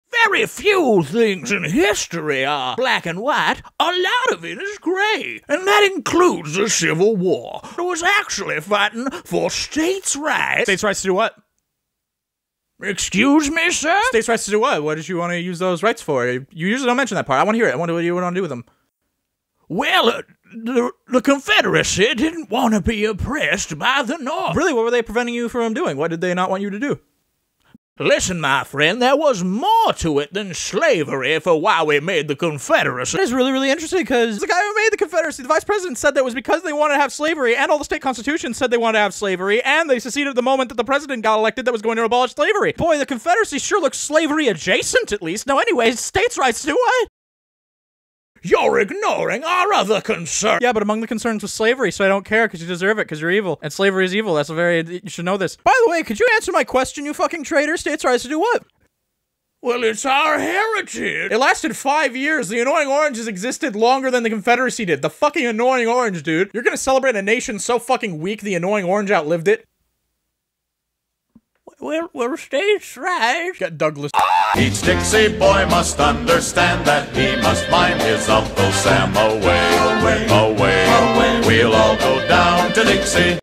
The Confederacy was Bad
I know I missed the 4th of july when this would have been fitting. But mocking confederates is a patriotic all year-'round!

also to anyone saying "The union was bad too". true, but they weren't the ones keeping slavery going lol.